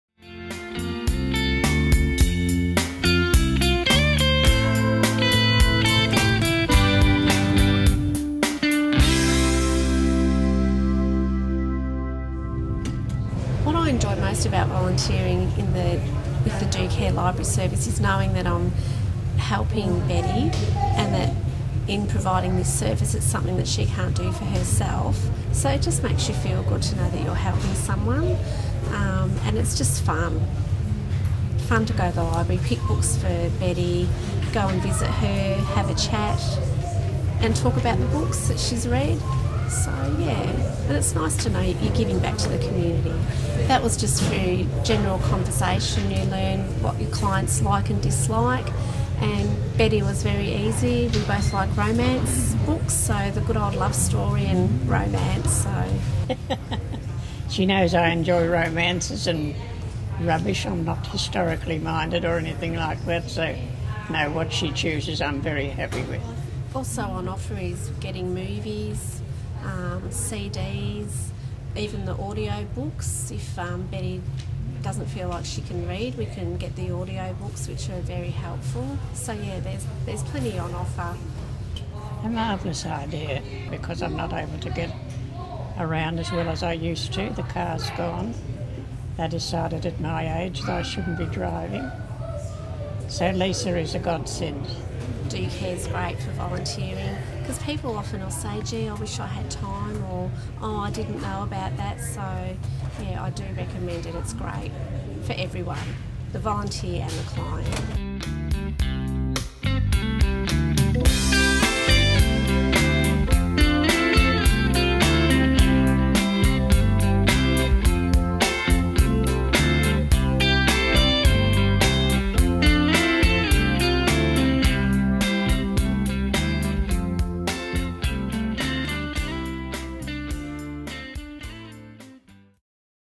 DoCare Geelong - Home Library Program
This service is available to older people who are unable to visit Geelong Regional Libraries due to frailty, temporary or permanent illness or disability.

This is a story about our clients and our volunteers.

Should you wish to receive this service or refer an older person to this
program, you may do so by contacting DoCare Geelong on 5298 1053.